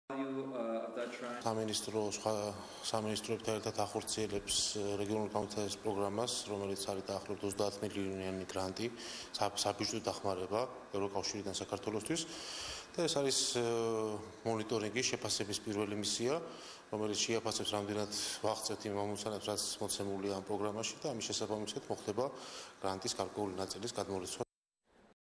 თენგიზ შერგელაშვილის კომენტარი
საქართველოს რეგიონული განვითარებისა და ინფრასტრუტურის მინისტრის მოადგილე, თენგიზ შერგელაშვილი საქართველოში ევროკავშირის წარმომადგენლობის ატაშესა და მისიის სხვა წარმომადგენლებს შეხვდა.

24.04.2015